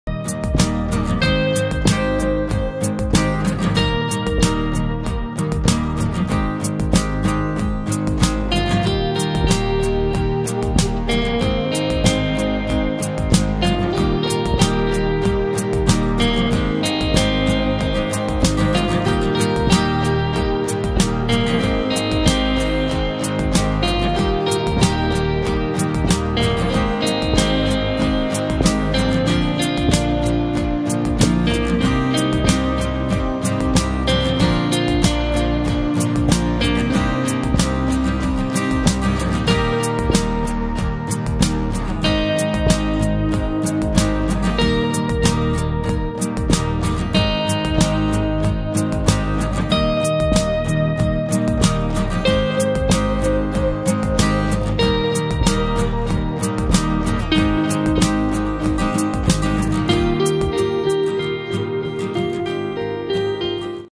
All home appliances that are a daily necessity new gadgets
Many tools are well organized with work to make life easy, simple but necessary
For everyday life and more gadgets in the next video.